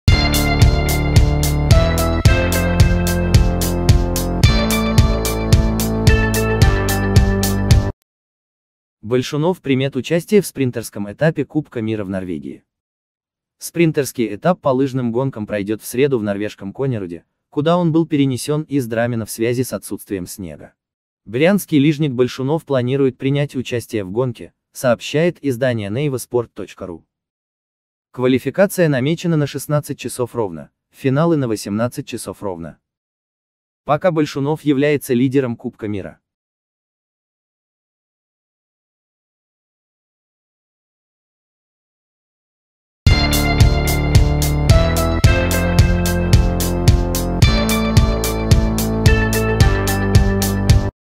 Большунов примет участие в спринтерском этапе кубка мира в Норвегии.
Спринтерский этап по лыжным гонкам пройдет в среду в норвежком Коннеруде, куда он был перенесен из Драммена в связи с отсутствием снега. Брянский лижник Большунов планирует принять участие в гонке, сообщает издание NevaSport.ru

Квалификация намечена на 16:00, финалы на 18:00.

Пока Большунов является лидером Кубка мира.